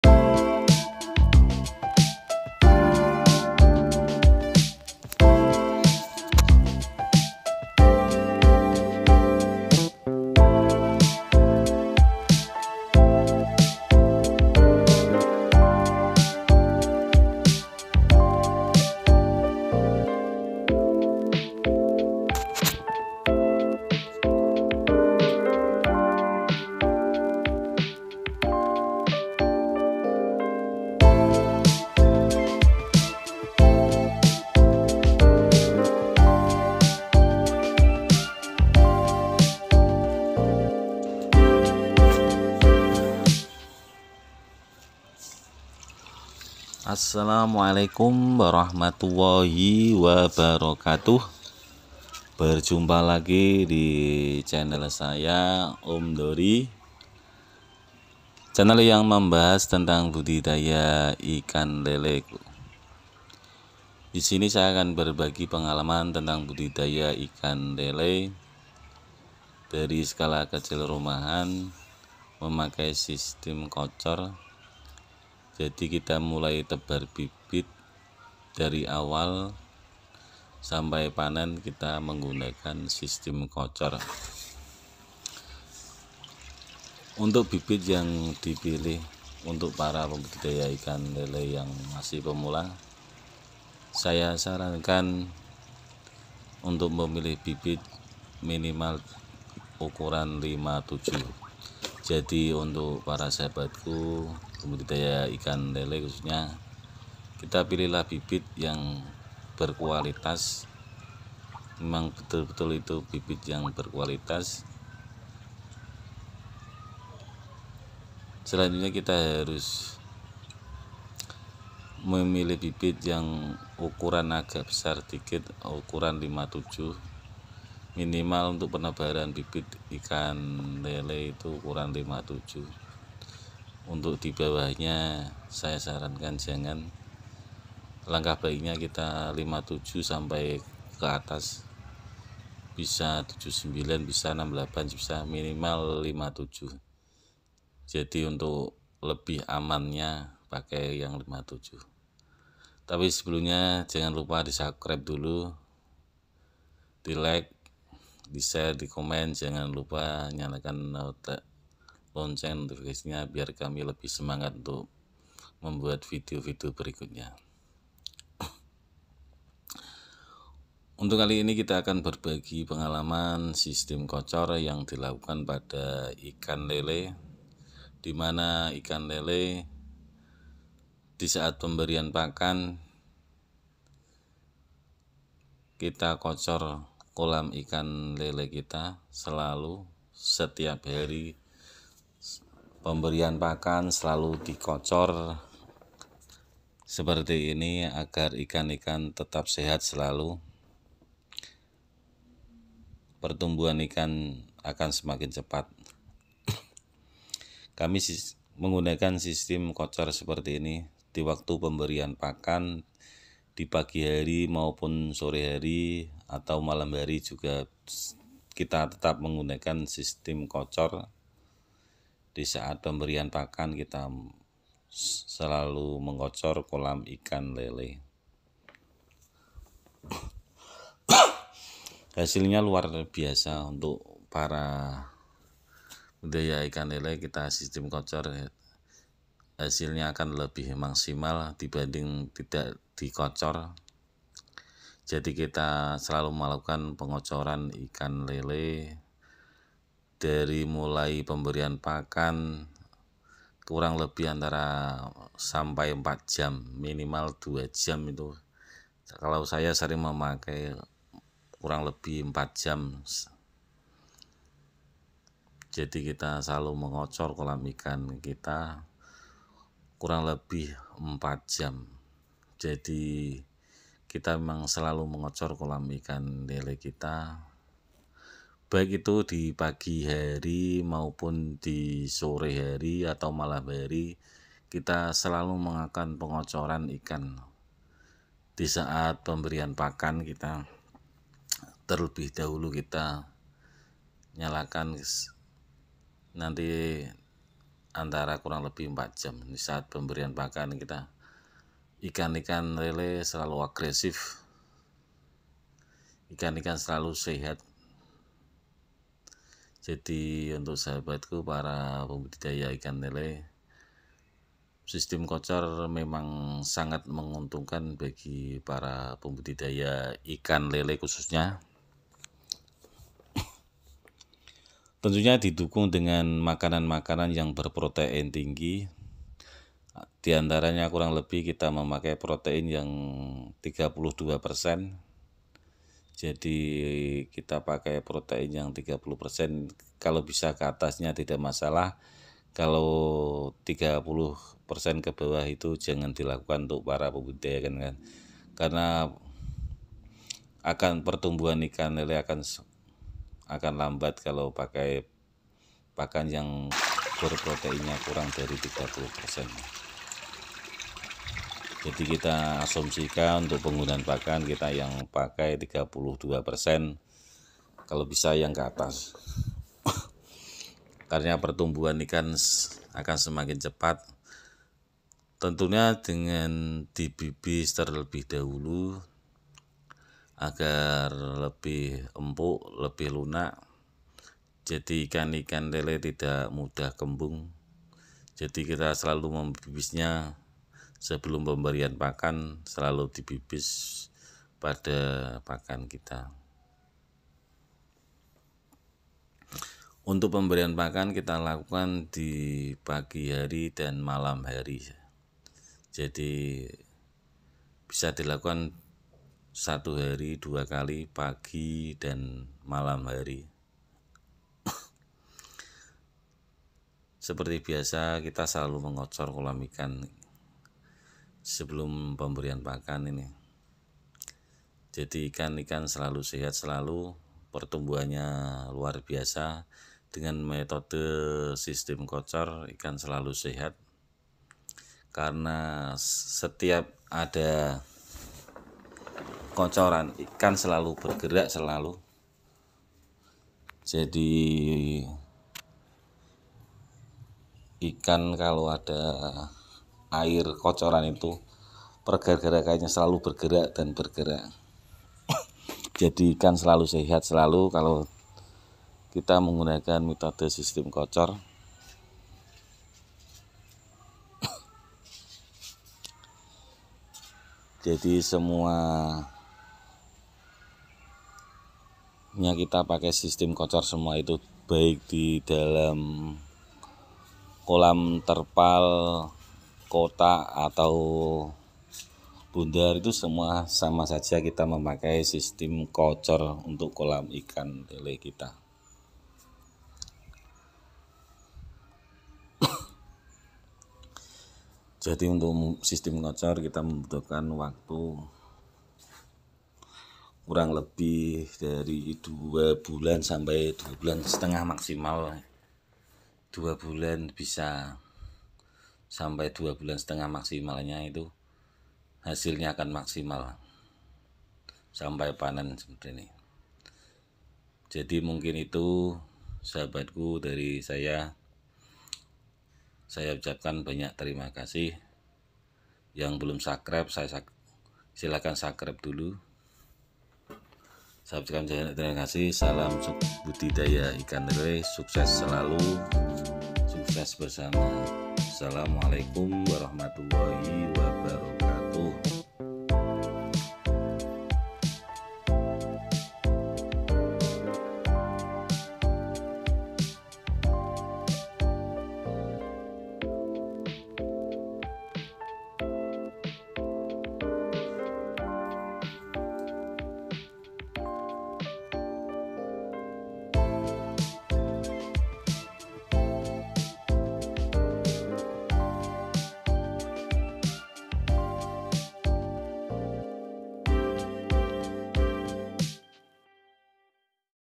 ternak lele sistem kocor ikan cepat besar
ternak lele menggunakan metode sistem kocor ikan lele pertumbuhan semakin cepat besar tentunya didukung dengan pemberian pakan ikan lele yang berprotein tinggi 32 persen pemberian pakan ikan lele selalu di Bibis pemberian pakan ikan lele dua kali sehari pagi dan malam hari